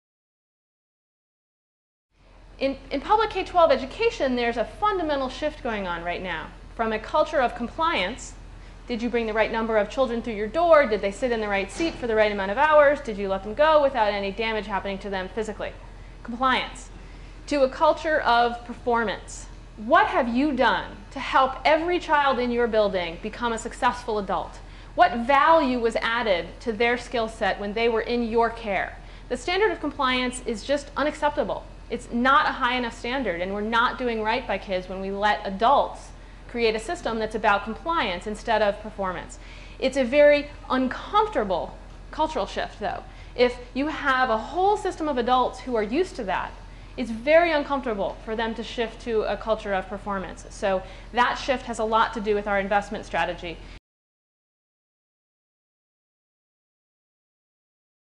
Kim Smith-Education: Shift From Culture of Compliance to C
There is a current shift in K-12 education from a culture of compliance, says Smith. Compliance is not a high enough standard and should not be accepted. Unfortunately, the culture shift to a new standard is uncomfortable since
      the old system